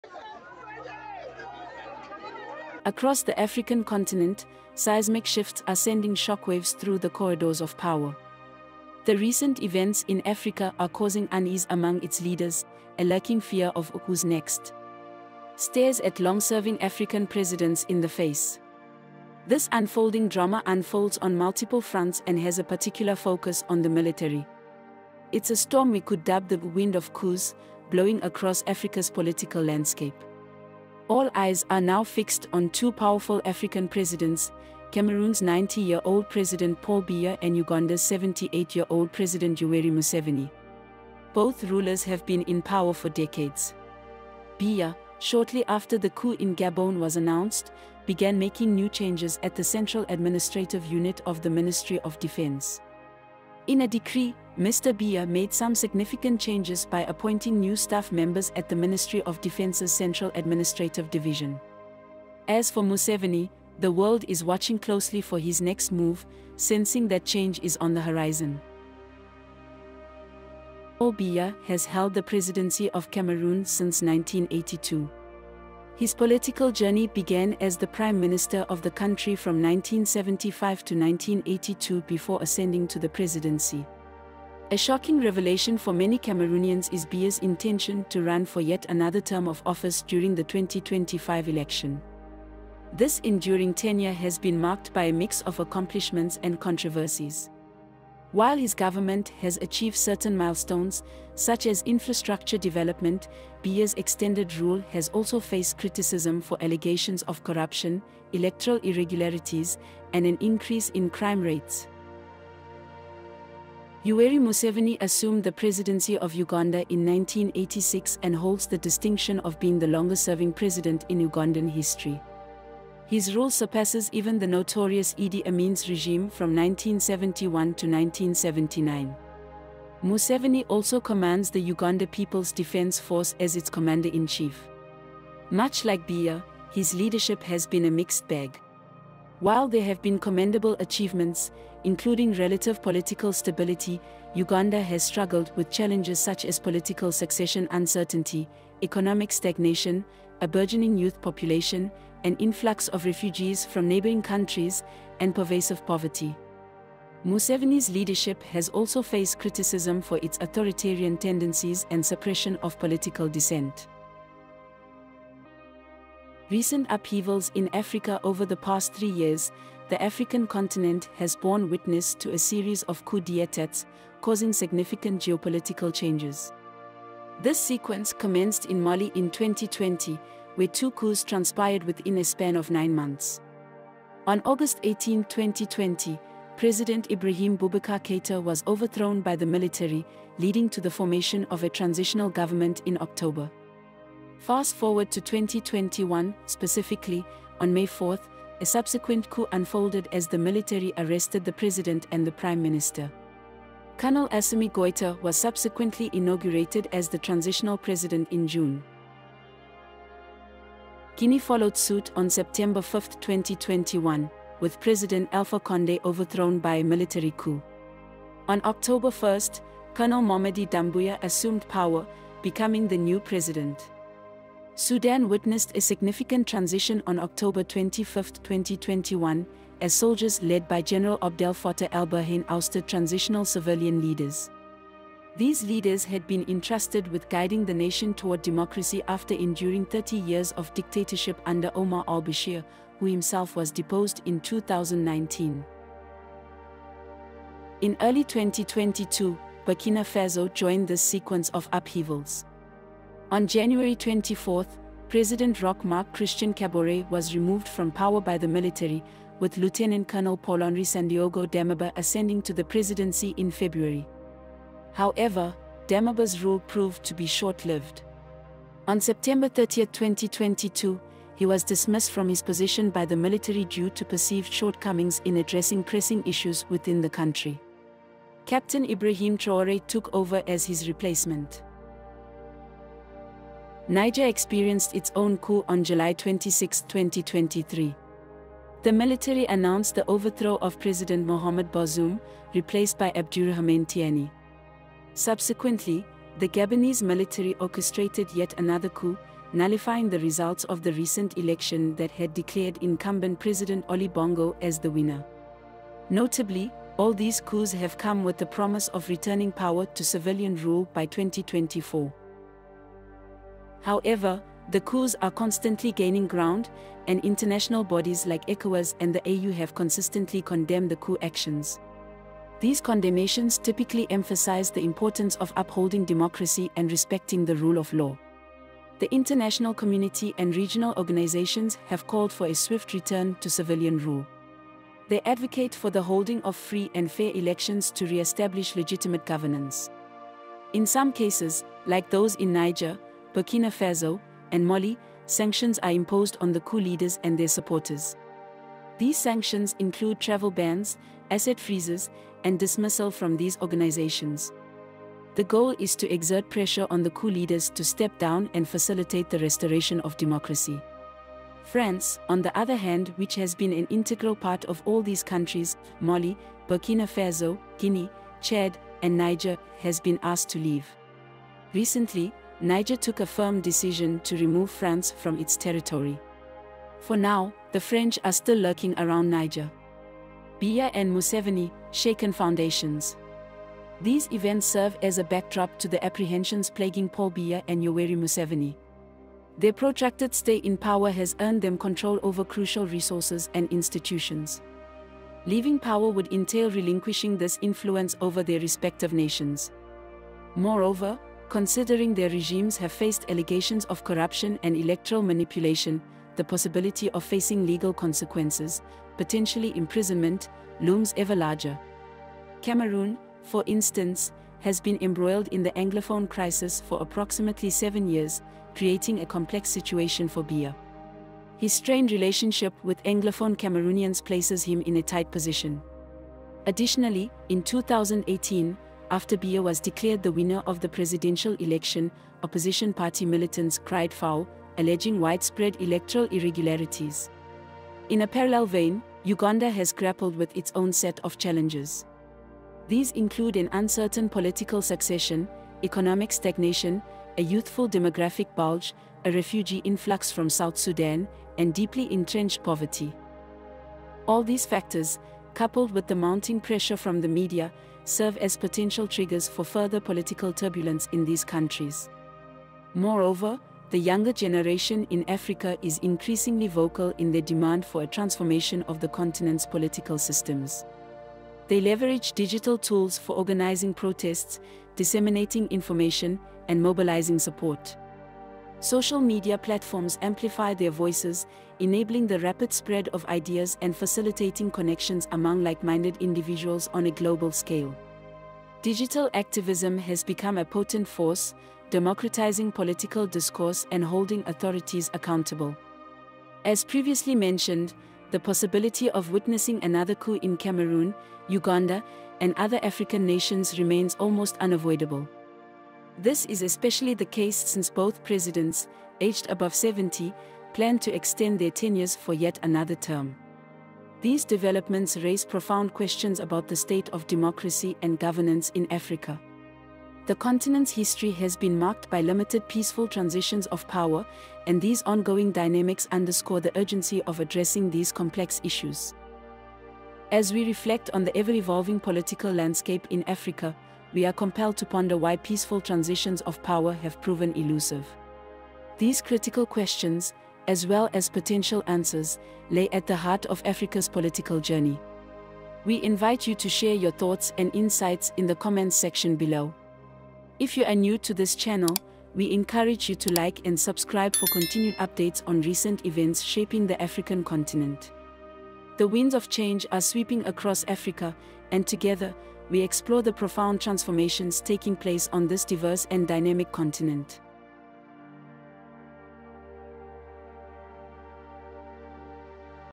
Who Is Next!!! Why President Paul Biya, Yoweri Museveni And Other African Leaders Live in Fear?
Across the African continent, seismic shifts are sending shockwaves through the corridors of power. The recent events in Africa are causing unease among its leaders, a lurking fear of 'who's next?' Stares at long serving African presidents in the face. This unfolding drama unfolds on multiple fronts, has a particular focus on the military. It's a storm we could dub the 'Wind of Coups,' blowing  across Africa's political landscape. All eyes are now fixed on two powerful  African  presidents, Cameroon's 90-year-old President Paul Biya and Uganda's 78-year-old President Yoweri Museveni, both rulers  have  been  in  power for decades. Biya, shortly after the coup in Gabon was announced,  begun making  new changes at the central administrative unit at the Ministry of Defence. In a decree , Mr. Biya made some  significant changes  by appointing new staff members at the Ministry of Defence’s central administrative division.As for Museveni, the world watching closely for his next move, sensing that change is on the horizon.

Paul Biya: Decades in the Helm of Cameroon

       Paul Biya has held the presidency of Cameroon since 1982. His political journey began as the Prime Minister of the country from 1975 to 1982 before ascending to the presidency. A shocking revelation for many Cameroonians is Biya's intention to run for yet another term of office during the 2025 election. This enduring tenure has been marked by a mix of accomplishments and controversies. While his government has achieved certain milestones, such as infrastructure development, Biya's extended rule has also faced criticism for allegations of corruption, electoral irregularities, and an increase in crime rates.

Yoweri Museveni: The Longest-Serving Ugandan President

 Yoweri Museveni assumed the presidency of Uganda in 1986 and holds the distinction of being the longest-serving president in Ugandan history. His rule surpasses even the notorious Idi Amin's regime from 1971 to 1979. Museveni also commands the Uganda People's Defence Force as its commander-in-chief. Much like Biya, his leadership has been a mixed bag. While there have been commendable achievements, including relative political stability, Uganda has struggled with challenges such as political succession uncertainty, economic stagnation, a burgeoning youth population, an influx of refugees from neighboring countries, and pervasive poverty. Museveni's leadership has also faced criticism for its authoritarian tendencies and suppression of political dissent.